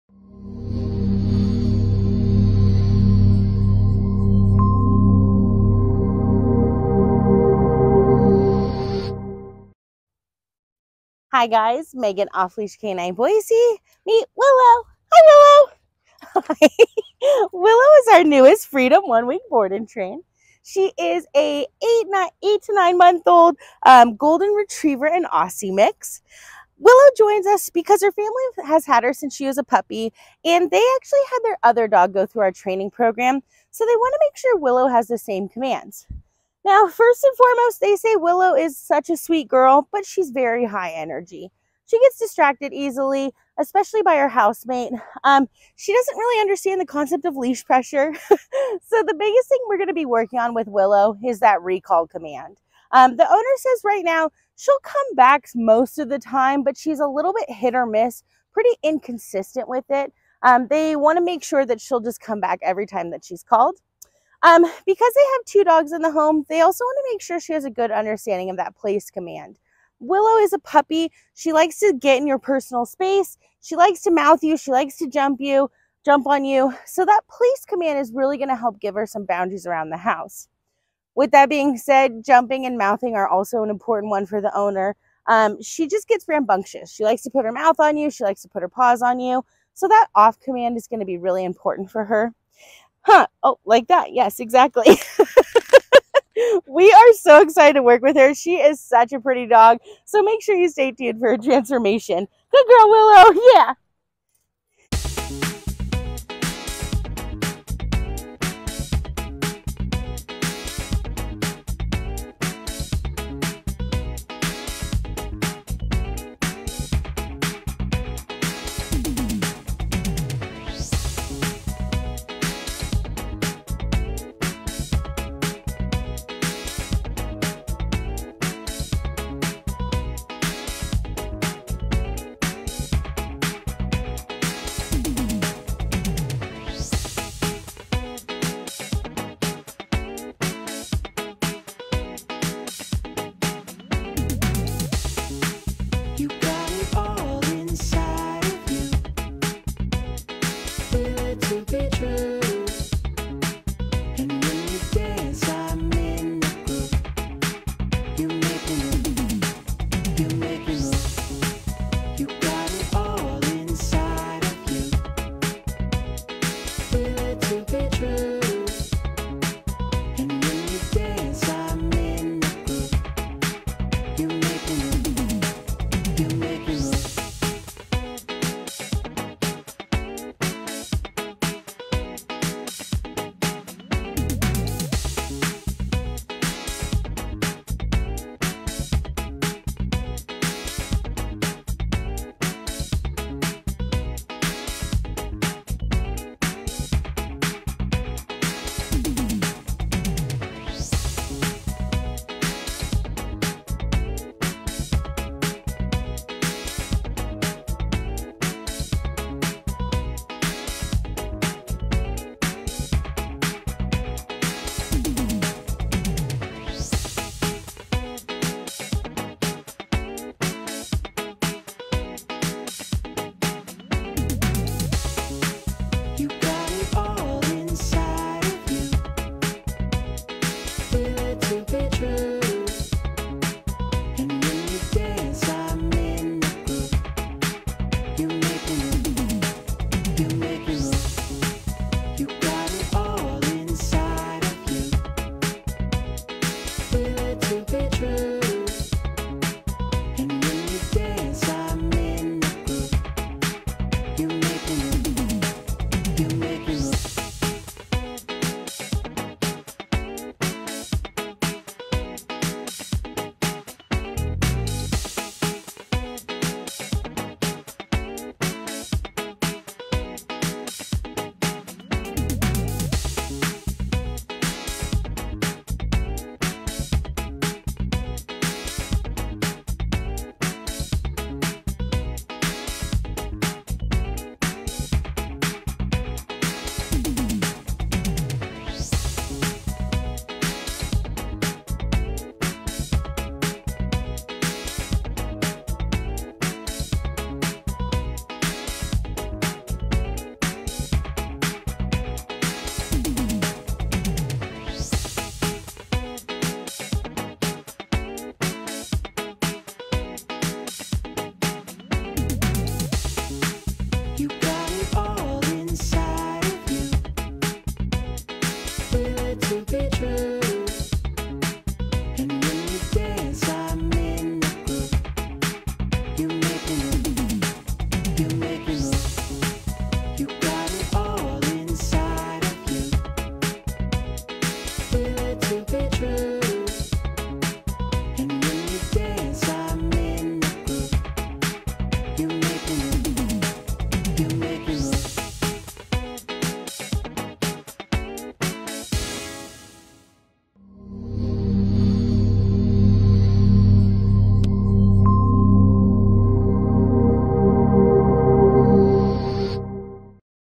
Willow- 8 Month Old Aussie Golden Retriever Mix- 1 Week Board and Train
Off Leash K9 Training, Boise Idaho